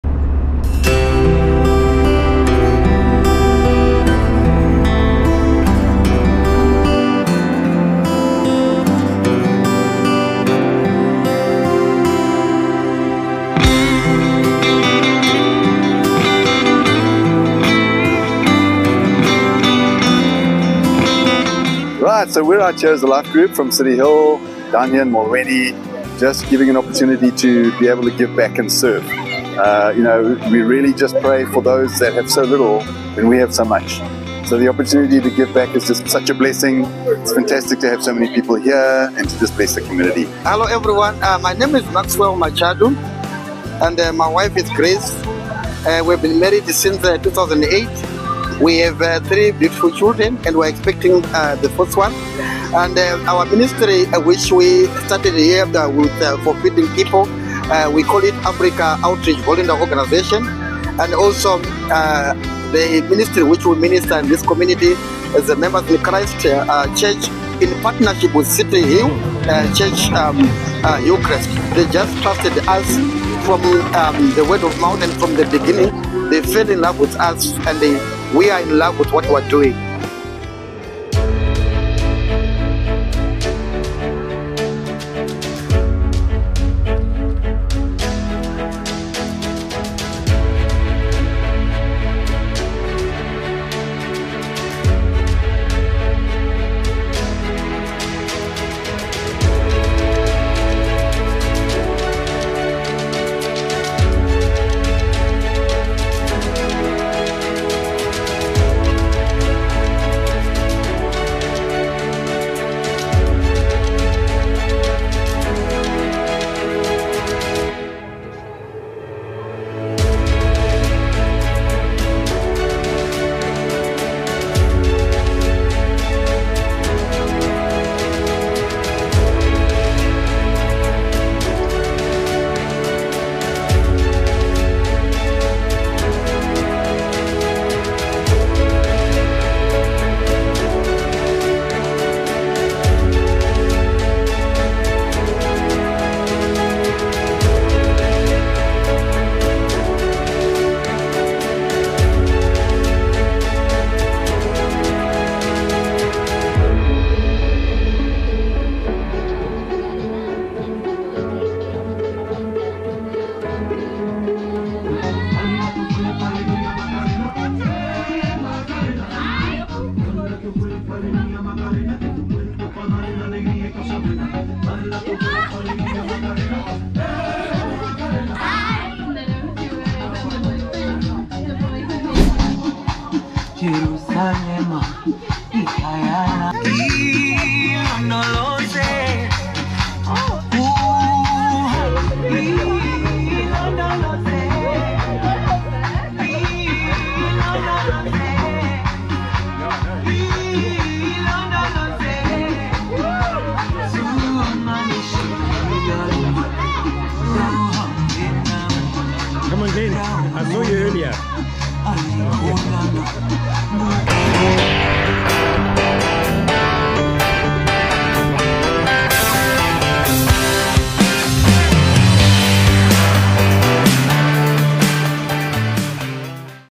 Cityhill Soup kitchen outreach to Molweni
Join us on a trip to Molweni for a soup kitchen outreach by a Cityhill Life Group.
Isaiah 58:10-14 Feed the hungry, and help those in trouble. Then your light will shine out from the darkness, and the darkness around you will be as bright as noon. The LORD will guide you continually, giving you water when you are dry and restoring your strength. You will be like a well-watered garden, like an ever-flowing spring. Some of you will rebuild the deserted ruins of your cities. Then you will be known as a rebuilder of walls and a restorer of homes. “Keep the Sabbath day holy. Don’t pursue your own interests on that day, but enjoy the Sabbath and speak of it with delight as the LORD’s holy day. Honor the Sabbath in everything you do on that day, and don’t follow your own desires or talk idly. Then the LORD will be your delight. I will give you great honor and satisfy you with the inheritance I promised to your ancestor Jacob. I, the LORD, have spoken!”